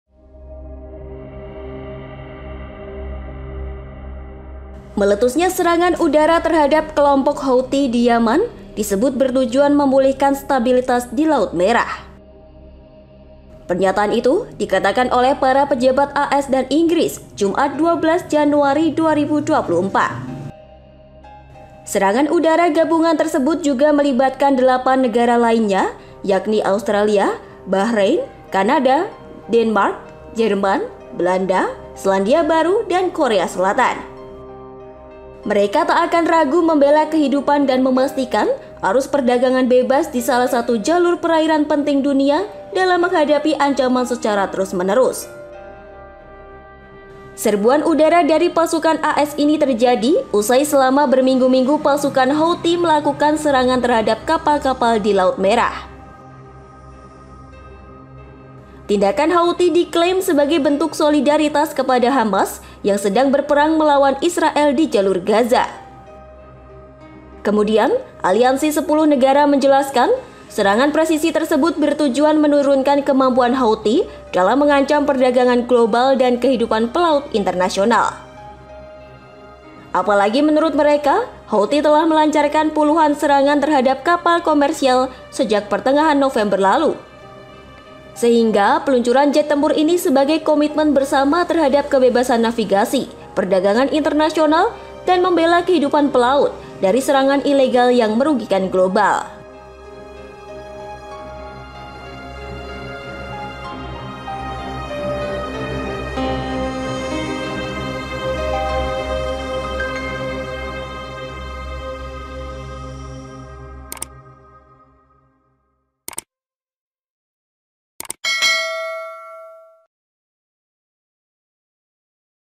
AS dan Inggris Jelaskan Tujuan Serang Houthi di Yaman
Amerika Serikat dan Inggris menjelaskan tujuan mereka menyerang Yaman, Jumat (12/1/2024).

AS, Inggris dan delapan negara lainnya menyebut serangan udara gabungan terhadap kelompok Houthi di Yaman bertujuan untuk memulihkan stabilitas di Laut Merah.

Selengkapnya dalam video berikut:

Penulis artikel: Irawan Sapto Adhi
Penulis naskah: Annisa Nurmaulia Al Fajri
Narator: Annisa Nurmaulia Al Fajri
Video editor: Galang Wahyu Permata
Produser: Dandy Bayu Bramasta